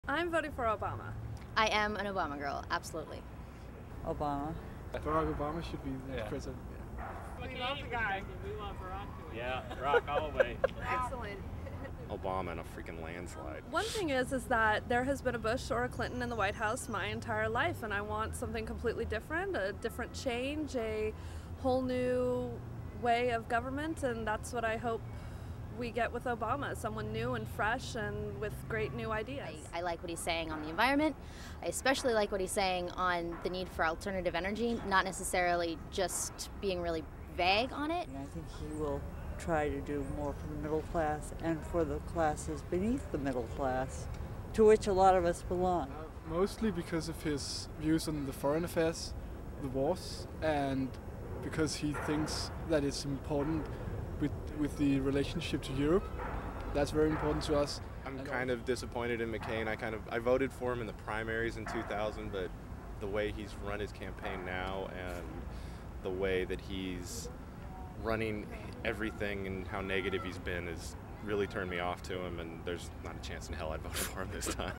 Everyone Loves Obama
A team of three reporters on Boston Common getting opinions from tourists and voters on who they believe should be the next president of the United States.  Everyone thinks Barack Obama is their man.